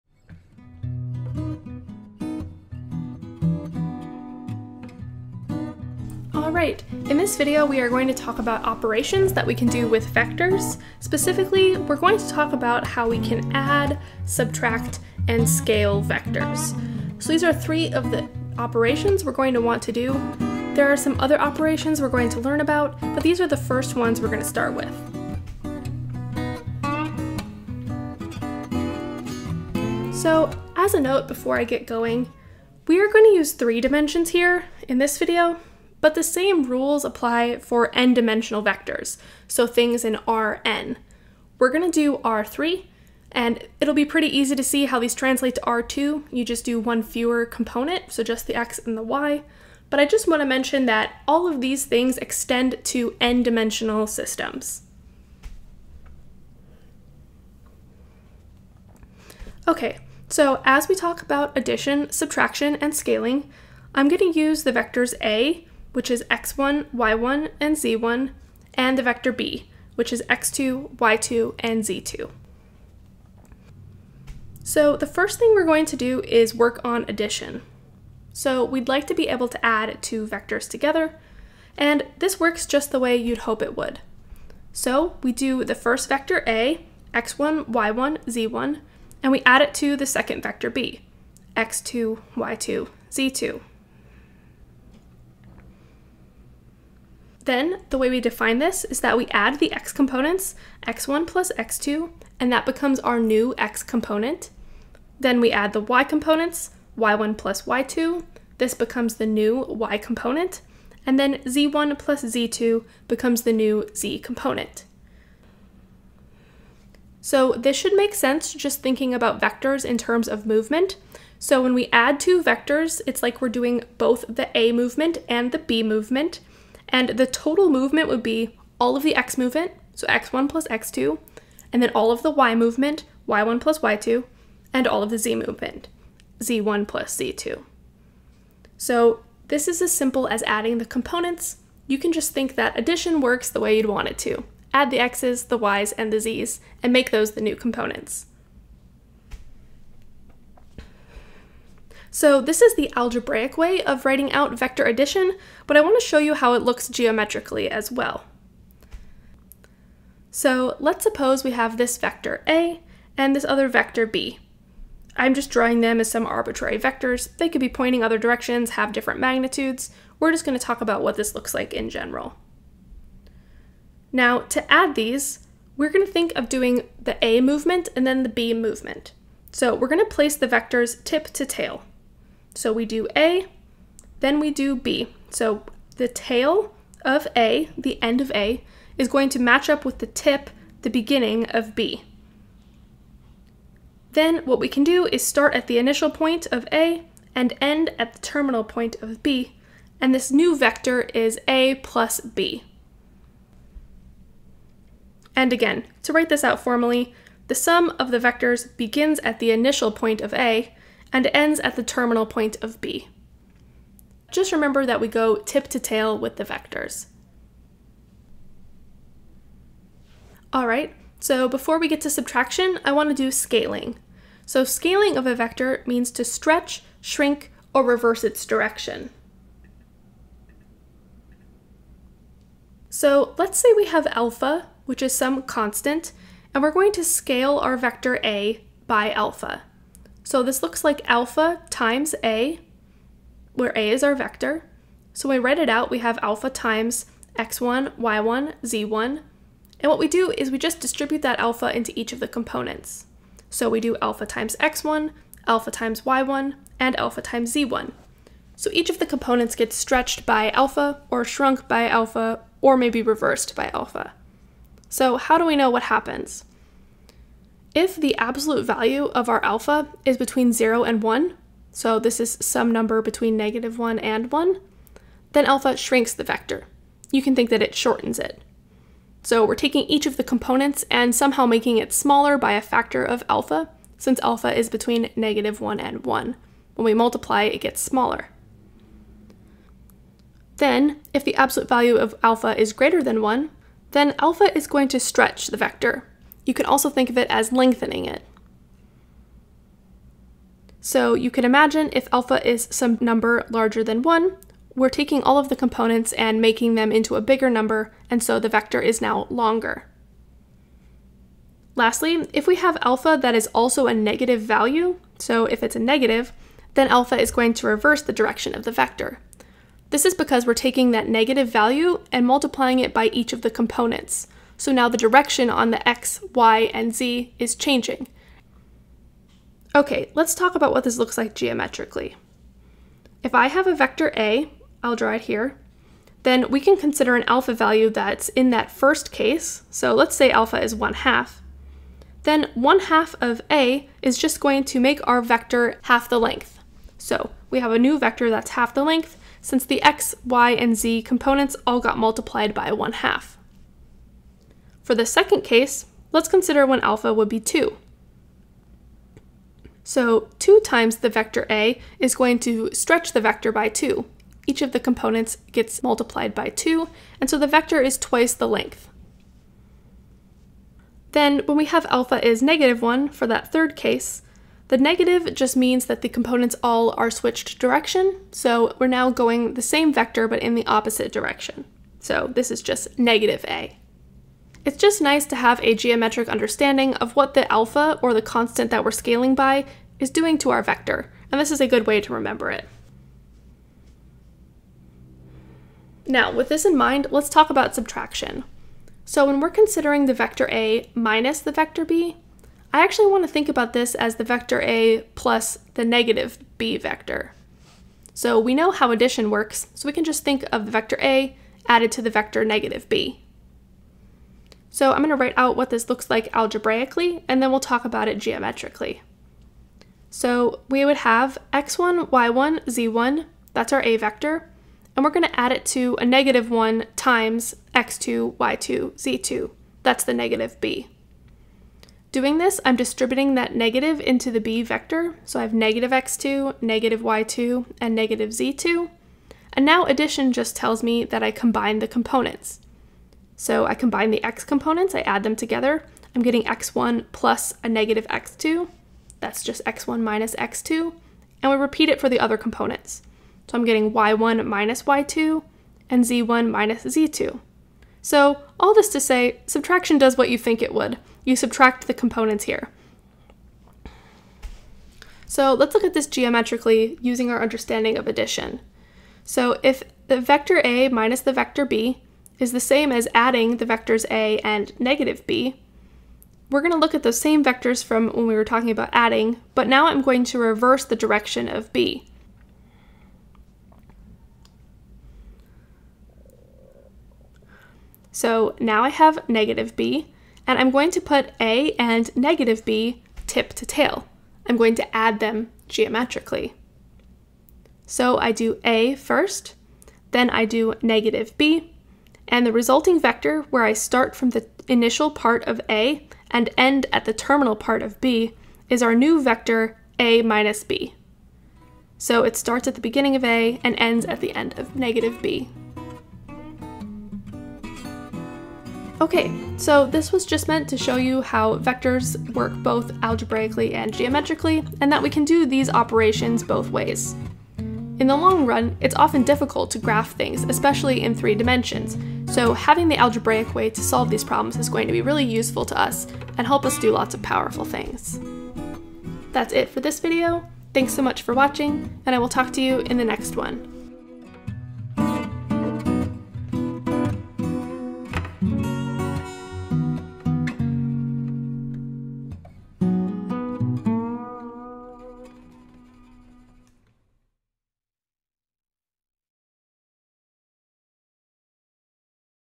Vector Operations: Addition, Subtraction, & Scaling
This video introduces the vector operations of adding, subtracting, and scaling. The example problem for this topic is in a separate video.